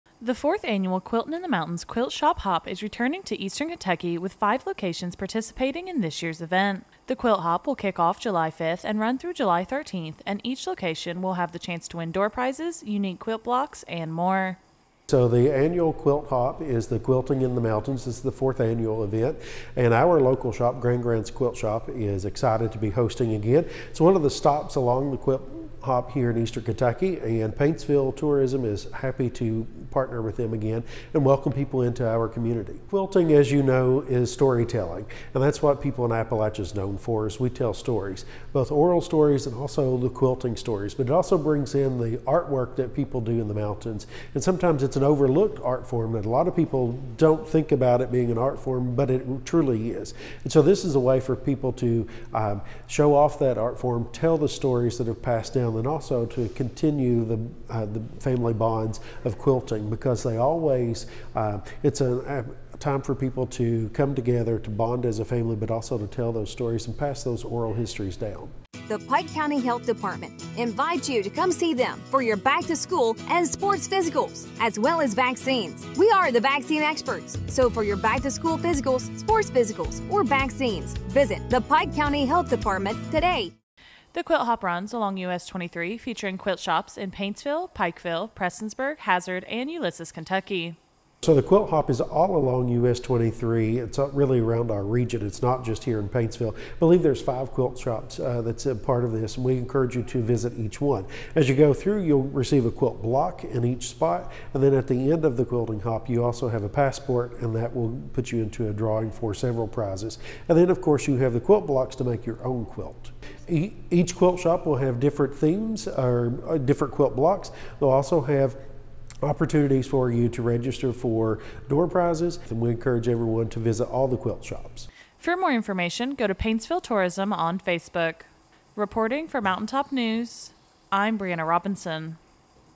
Quiltin' in the Mountains Returns Beginning July 5, 2024
The fourth-annual Quiltin' in the Mountains Shop Hop will be returning to the region beginning July 5, 2024.